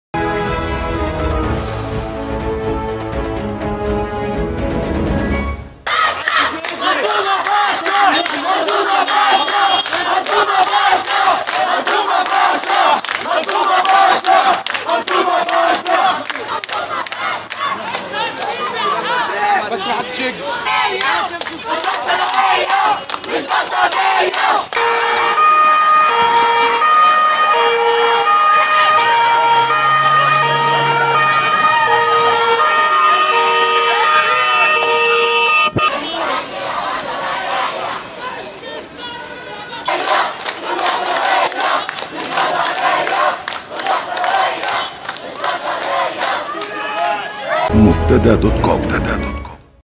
مطالبين بإقالة الشربينى.. طلاب الثانوية يتظاهرون أمام وزارة التعليم
تظاهر طلاب وطالبات الثانوية العامة وأولياء أمورهم أمام مقر وزارة التربية والتعليم ضد قرارات  وزير التعليم الهلالى الشربينى، بإلغاء امتحان مادة "الديناميكا" بعد تسريبه وتأجيل امتحانات الجيولوجيا والتاريخ والجبر للأسبوع المقبل.